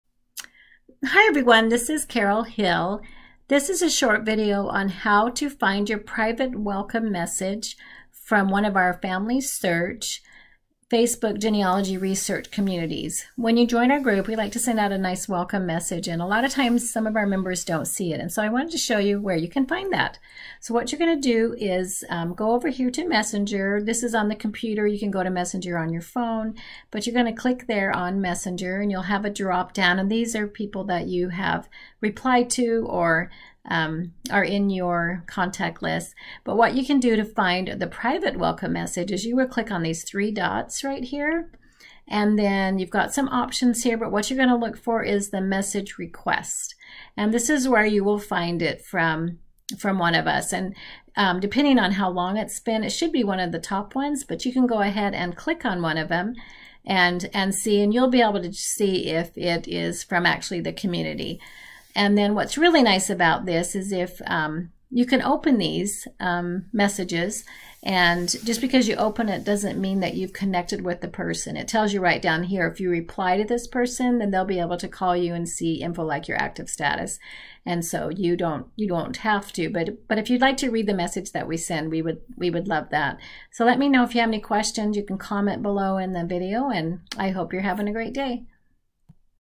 How to Find your Private Welcome Message on in Messenger
When you join one of our FamilySearch Facebook Genealogy Communities, we send you a private message.  If you haven't seen it, watch this video to learn how to find it.